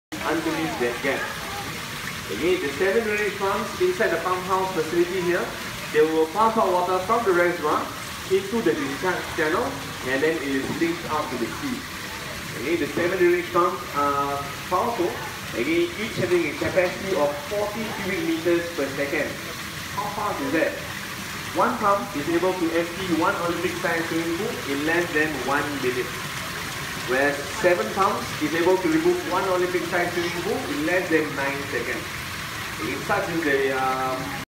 Use of water pumps in the Marina Barrage to alleviate flooding
Another type of flood alleviation mechanism utilized in Singapore.